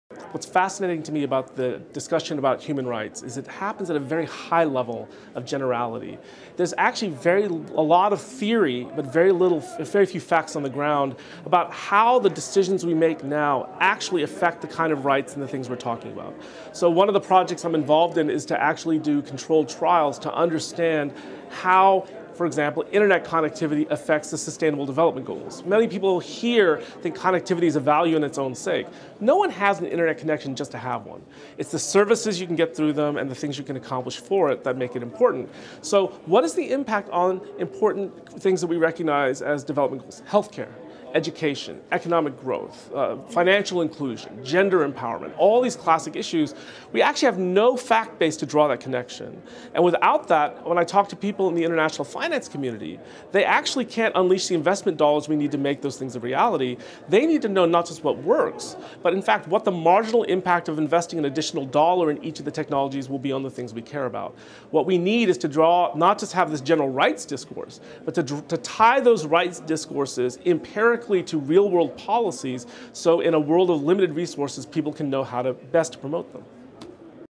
Global IGF 2018: Christopher Yoo on How to Enhance Global Cooperation in the Digital Age
Christopher Yoo, professor, University of Pennsylvania, United States, shares their answer to the question, “What will bring people together for better cooperation on digital issues-- what mechanisms might be employed? Should the UN Declaration of Human Rights be updated to include Digital Rights?” Recorded at the Global Internet Governance Forum 2018 in Paris, France, by the Imagining the Internet Center, an initiative of Elon University, North Carolina, USA.